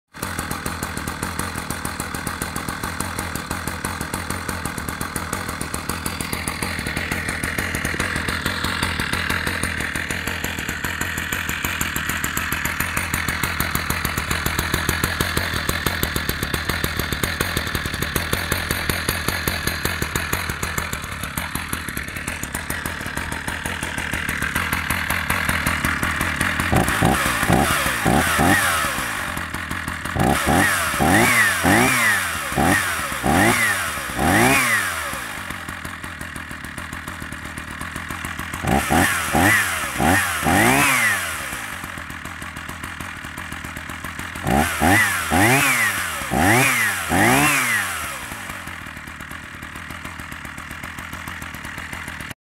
Husqvarna cd480 első indítása
Husqvarna cd480 felújítás utáni első indítás, még nincs beállítva teljesen.